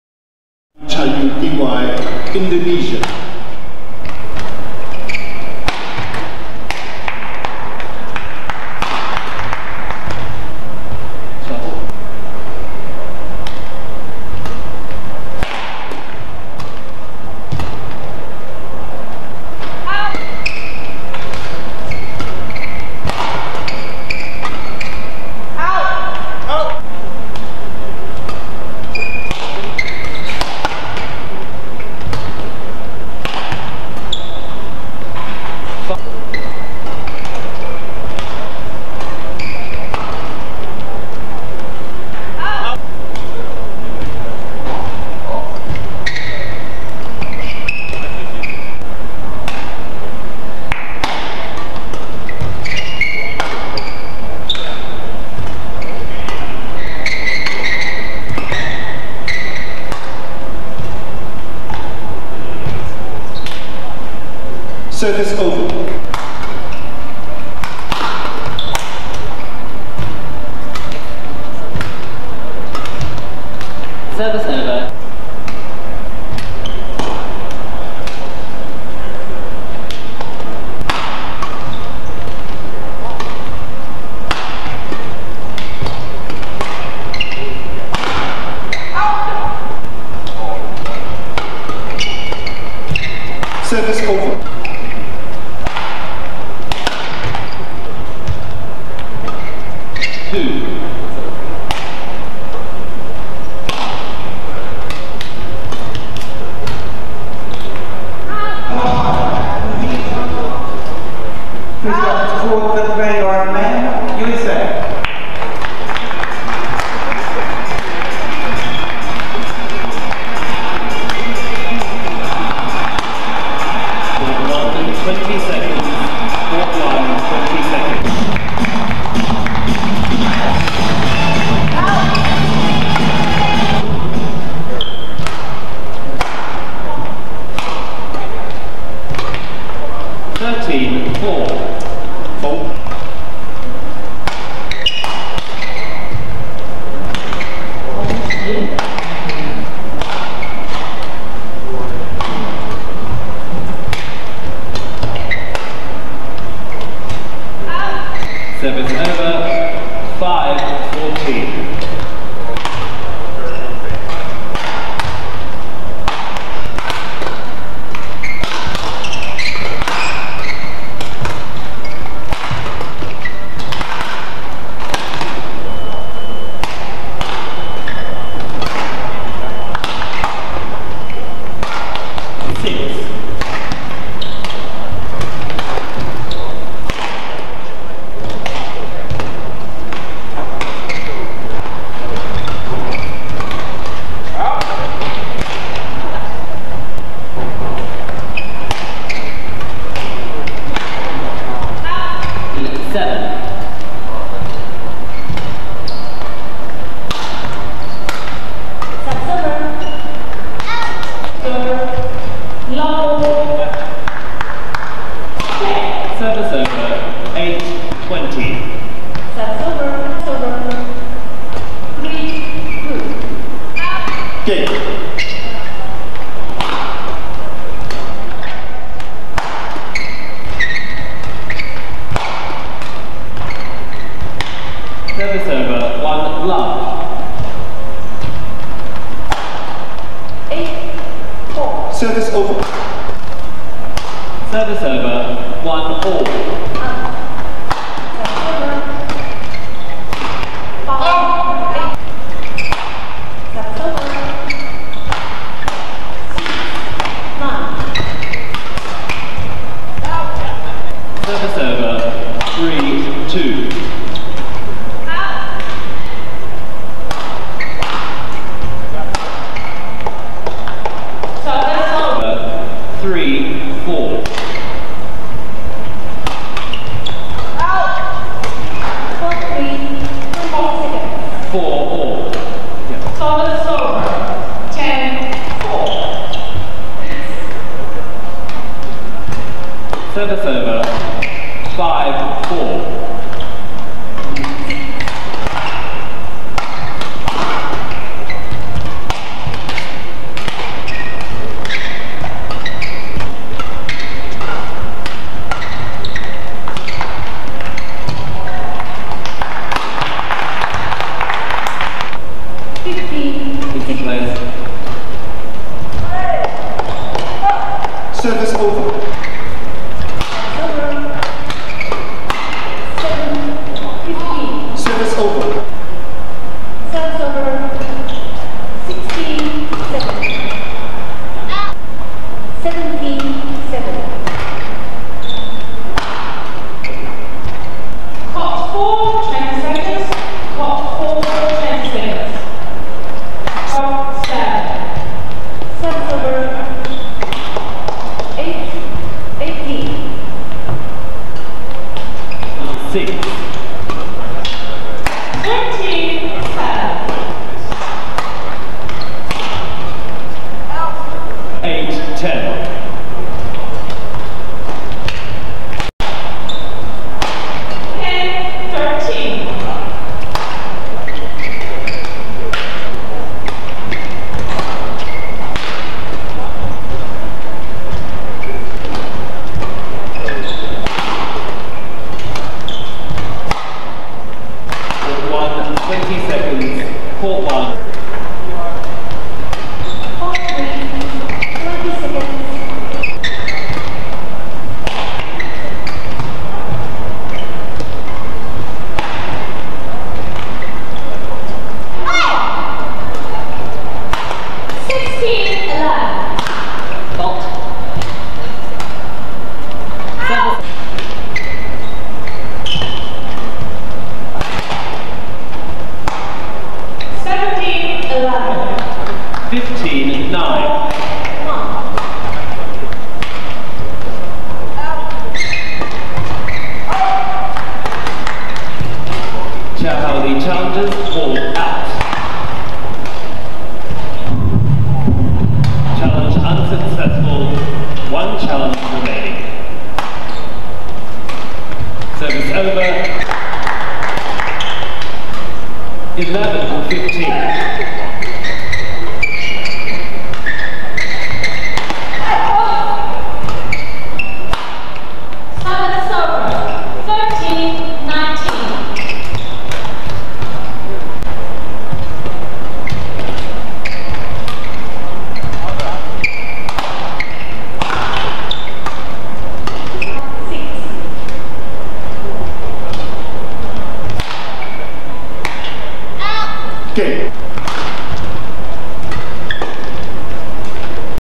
Komang Ayu Cahya Dewi vs Victoria Tjonadi || R32 Australia Open 2024
Sathio Australia Open 11-16 Juni 2024
Round Of 32:
Komang Ayu Cahya Dewi (INA) vs Victoria Tjonadi (AUS)